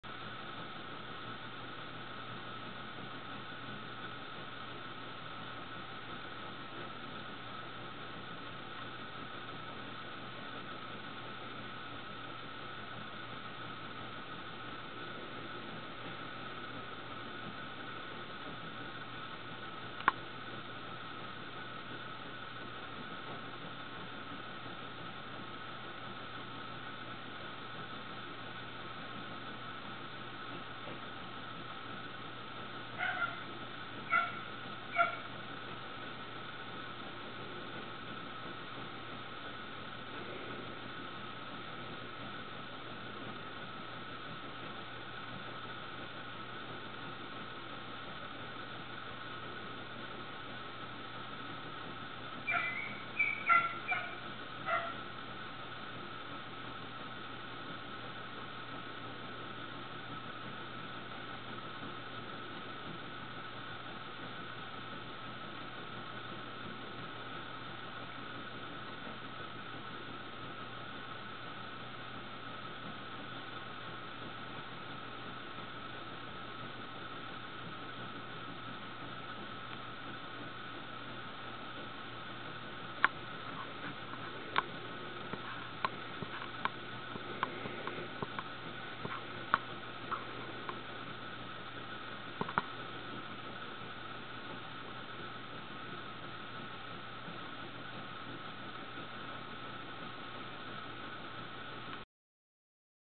Scotties still chasing squirrels
The squirrels are still at it and the scotties are ever so vigilant.